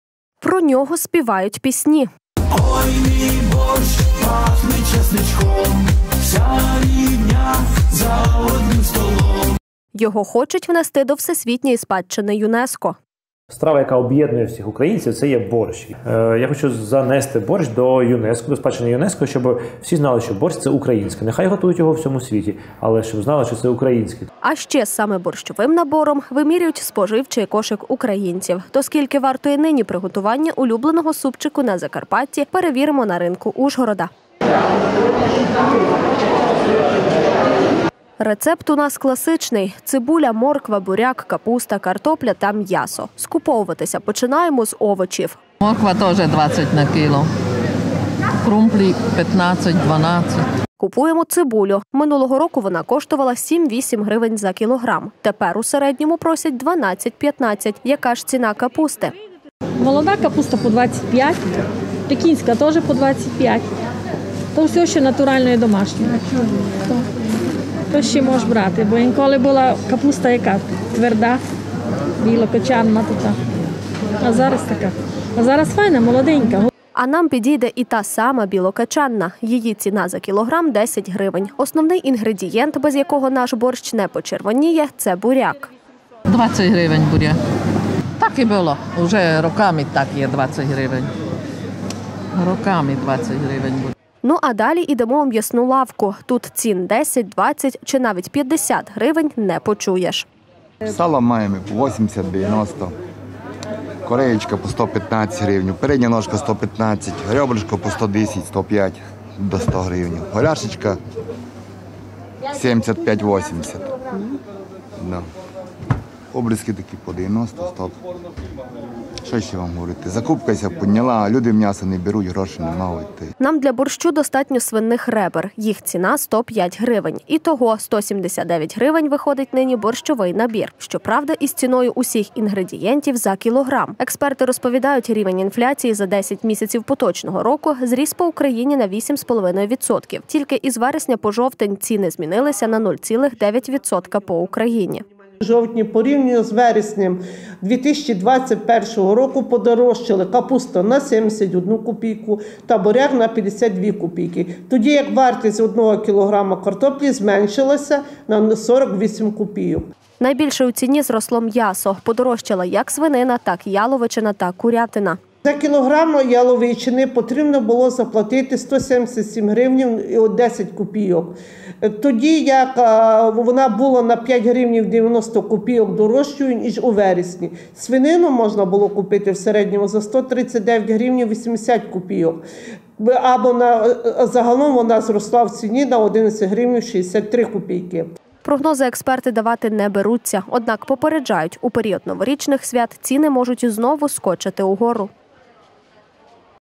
Дорогий борщ? Скільки вартує борщовий набір?
Його готують всі, він об’єднує Українців, а склад його продуктів допомагає статистам вирахувати рівень інфляції. Нині існує майже сотня рецептів борщу. Мої колеги взяли класичний рецепт і вирушили на ринки Ужгорода, аби дізнатися, скільки коштує нині приготувати борщ. Про нього співають пісні. Його хочуть внести до Всесвітньої спадщини ЮНЕСКО. А ще саме борщовим набором вимірють споживчий кошик українців. То скільки вартує нині приготування улюбленого супчику на Закарпатті перевіримо на ринку Ужгорода. Рецепт у нас класичний: цибуля, морква, буряк, капуста, картопля та м'ясо. Скуповуватися починаємо з овочів. Купуємо цибулю. Минулого року вона коштувала 7-8 гривень за кіло. Тепер – у середньому просять 12-15-ть. Яка ж ціна капусти? А нам підійде і та сама білокачанна – її ціна за кілограм – 10 грн. Основний інгредієнт без якого наш борщ не почервоніє – буряк. Ну, а далі ідемо у м’ясну лавку. Тут цін 10, 20 чи навіть 50 грн не почуєш. Нам для борщу достатньо свинних ребер їх ціна 105 гривень. І того: 179 гривень виходить нині борщовий набір. Що правда з ціною всіх інгредієнтів за кілограм. Експерти розповідають рівень інфляції за 10 місяців поточного року зріс по Україні на 8,5 відсотків. Тільки з вересня по жовтня ціни змінилися на 0,9 % по Україні. Найбільше у ціні зросло м'ясо. Подорожчала як свинина, так яловичина та курятина. Прогнози експерти давати не беруться, однак попереджають, що в період новорічних свят ціни можуть знову скочити в гору.